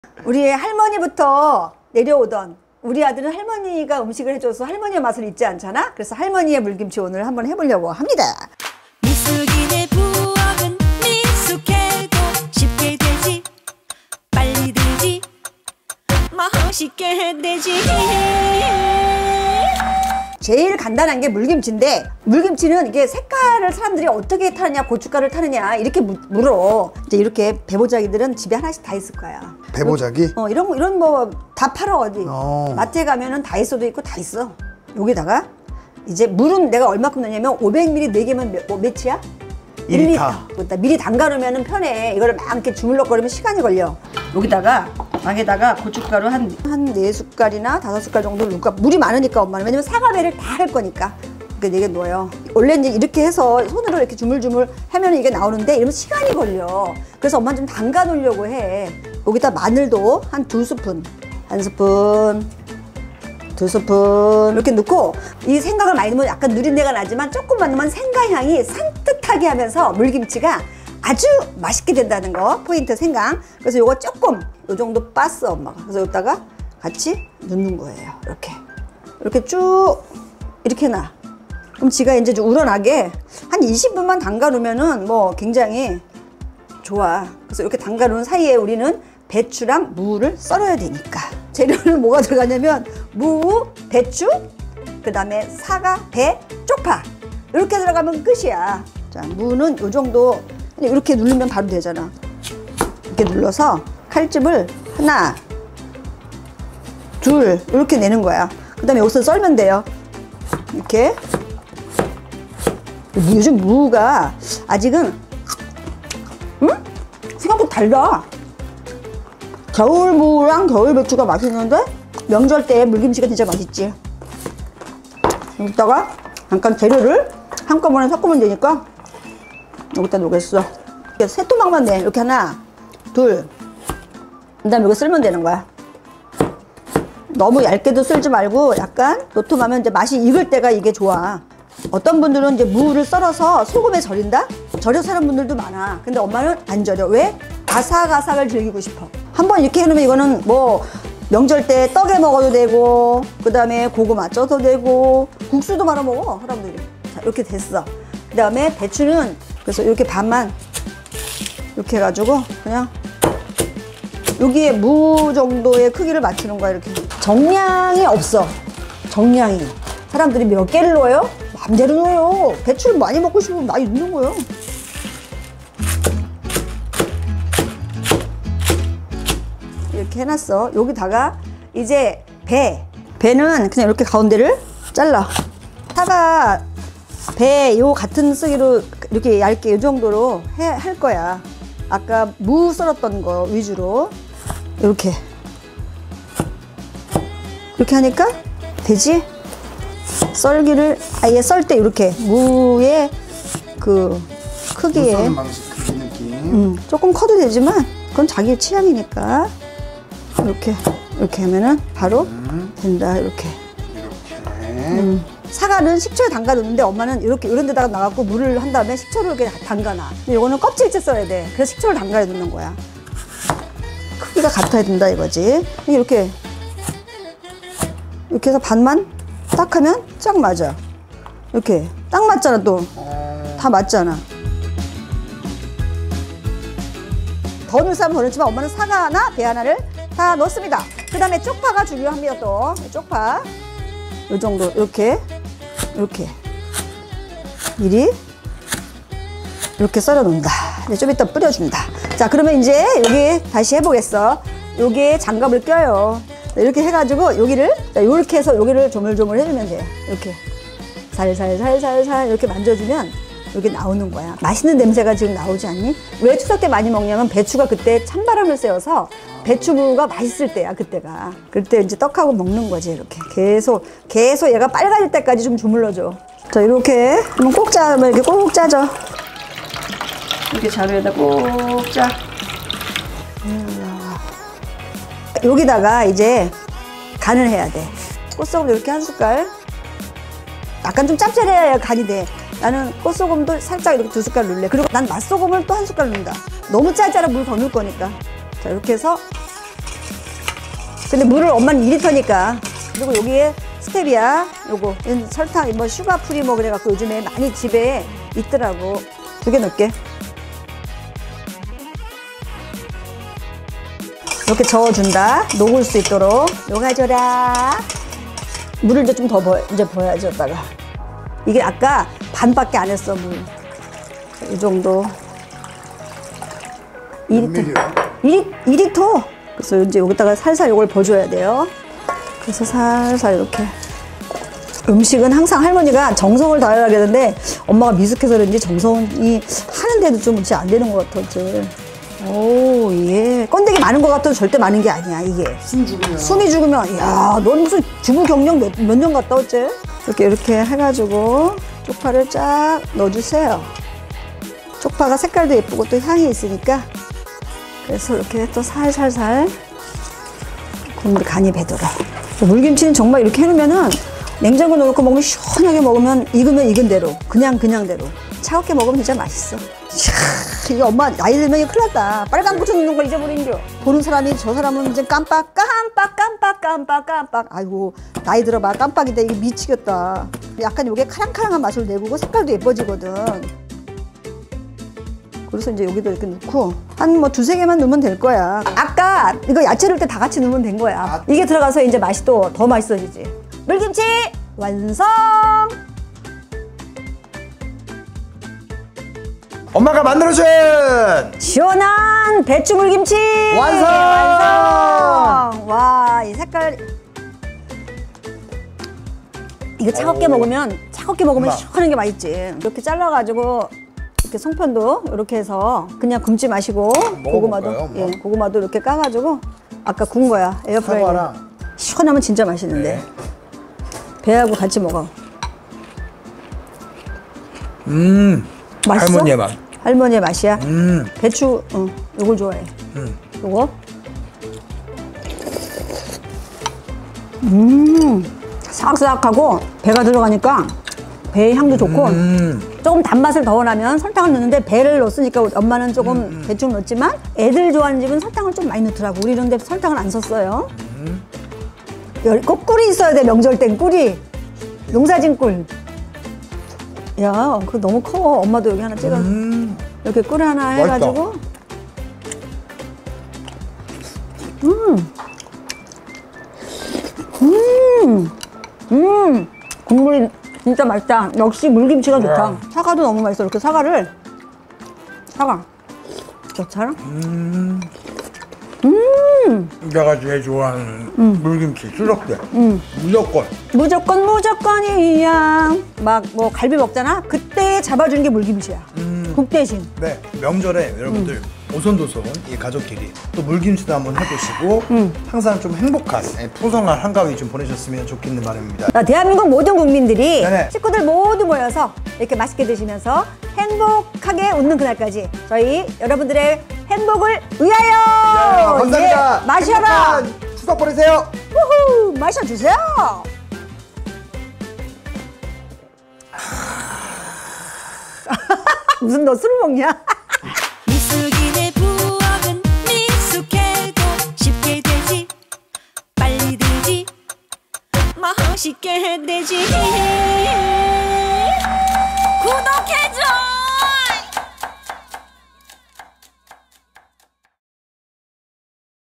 썰기만 하면 끝! 찬 바람 불 땐 초간단 배추 물김치~!
[미숙한 부엌 시즌2] ep.44 썰기만 하면 끝! 찬 바람 불 땐 초간단 배추 물김치~!

여러분, 오늘도 다가온 추석에 딱! 맞는 요리를 준비했어요~
송편, 고구마에 곁들여 먹어도 좋고
소면을 준비해서 넣어 국수처럼 먹어도 좋은 초간단 물김치랍니다!
미숙언니와 김PD에게는 추억의 음식이기도 해요~

식구들끼리 모여앉아 함께 야채손질하고
송편과 함께 시원~한 물김치 먹으며 이번 추석 보내보면 어떨까요?
**물김치: 고추가루4, 다진 마늘2, 다진 생강 0.5
 무 반개, 배추 양껏, 배 1개, 사과 1개, 쪽파 반단, 홍고추 1개
꽃소금2, 맛소금1, 스테비아2포